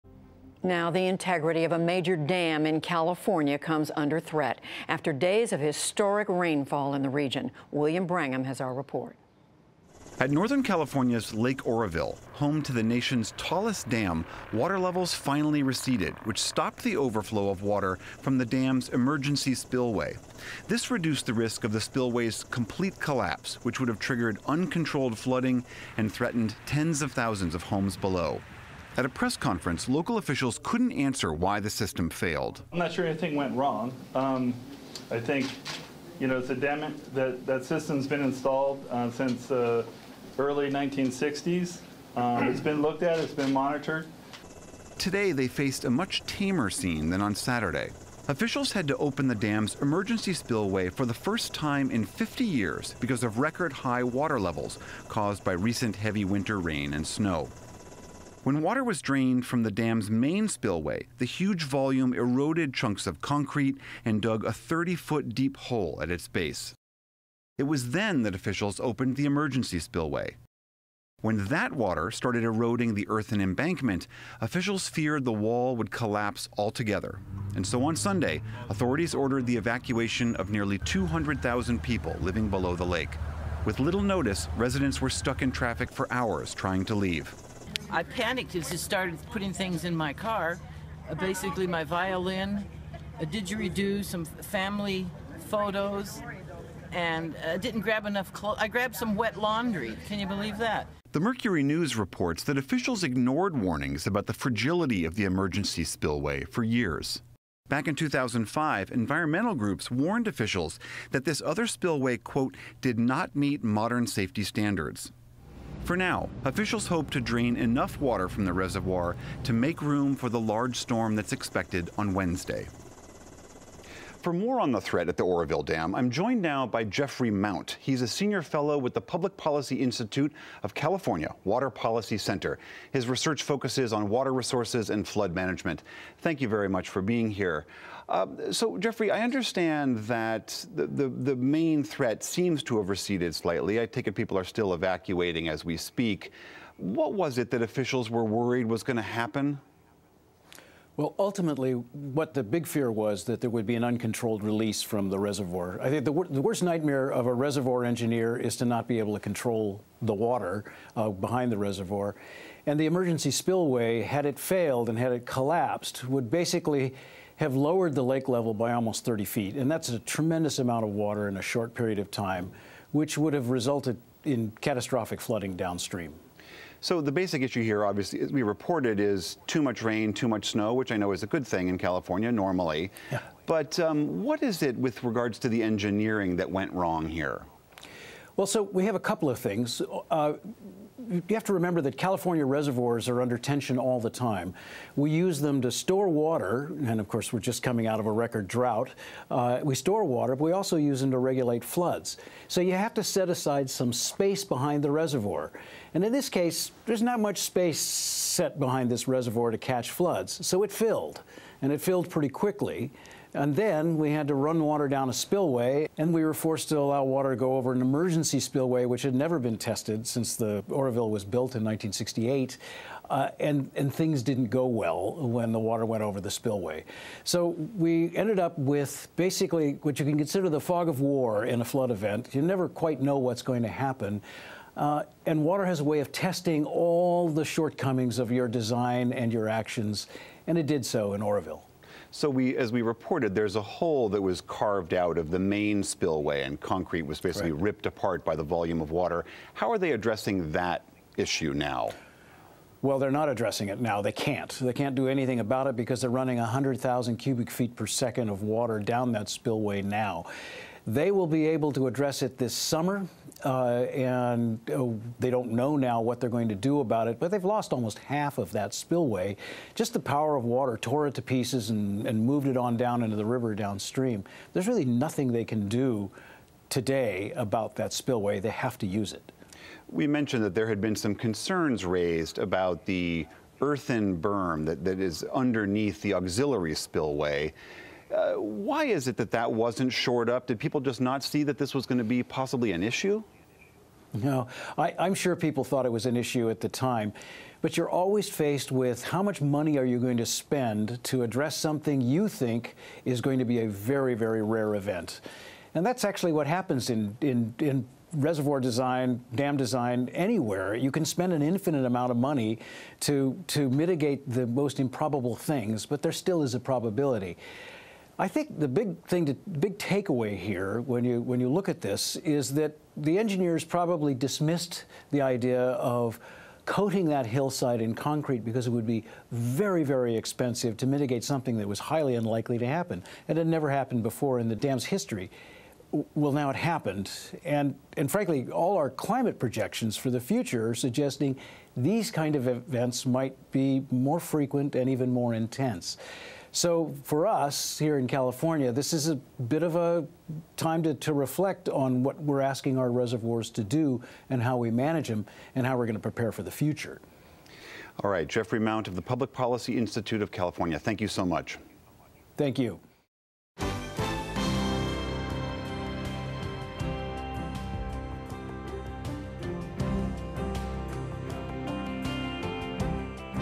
Precarious Oroville Dam highlights challenges of California water management
At Northern California’s Lake Oroville, water levels receded Monday, stopping the overflow of water from the dam’s emergency spillway. This reduced the risk of immediate uncontrolled flooding -- but longer-term concerns remain. William Brangham speaks with Jeffrey Mount of the Public Policy Institute of California about the massive evacuation that took place and the outlook for the dam's future.